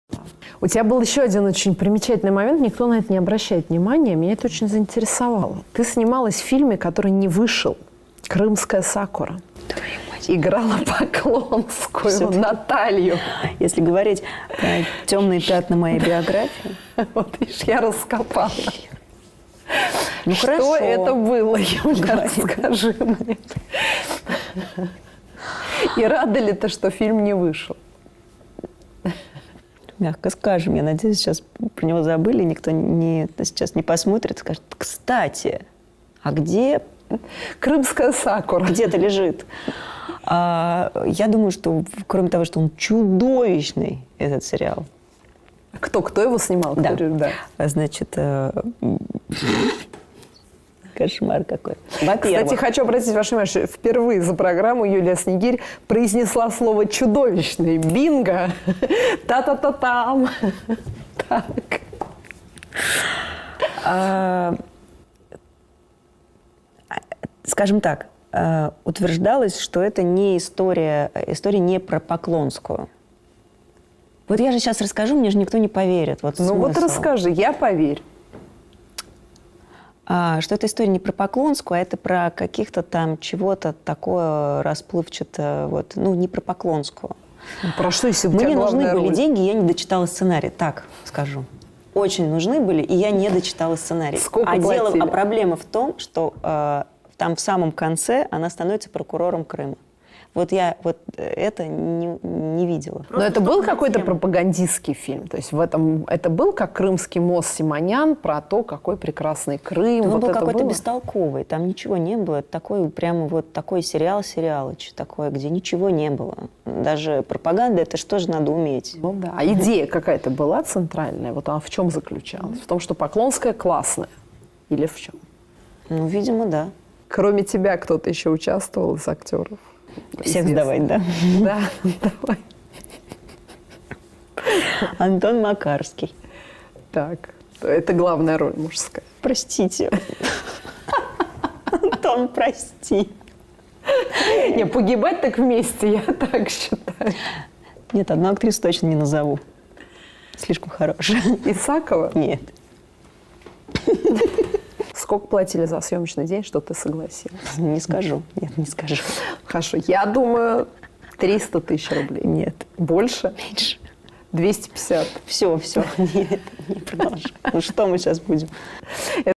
Юлия Снигирь о своей самой позорной роли
Юлия Снигирь рассказала о своей самой позорной роли.